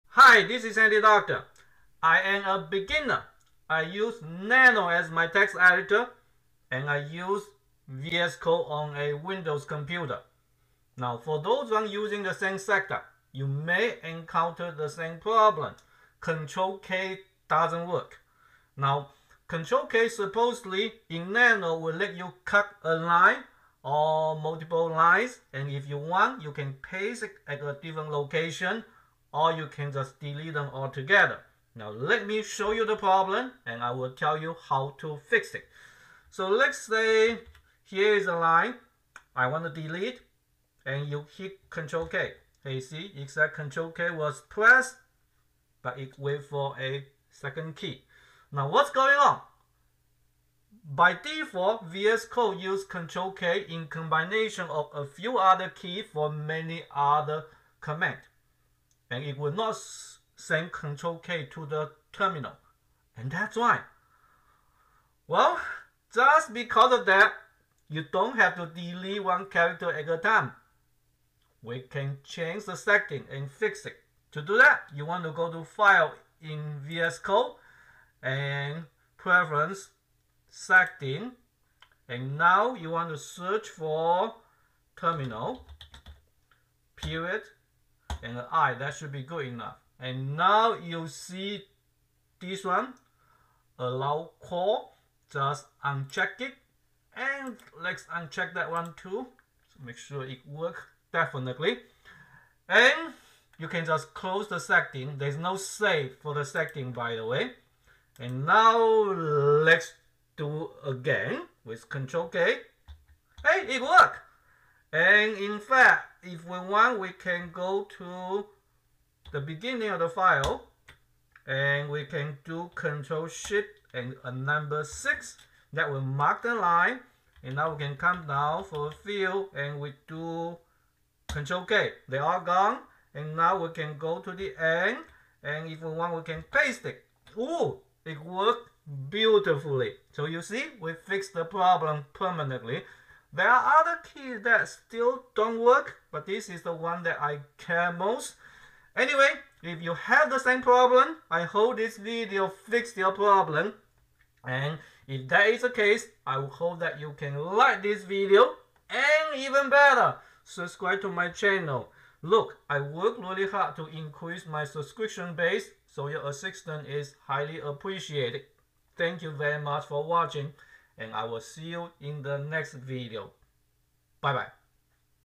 ctrl K doesn't work in Nano under VS code. How to fix it. Nano text editor, Linux, terminal
If you use Nano as your text editor like me and happen to use VS code together, oh well, ctrl K doesn't work for Nano. So you can't delete lines with ctrl K?! We can fix it.

If you are wondering, the whole reason we even use Nano is because we just made a website and are still making it better. Thank you.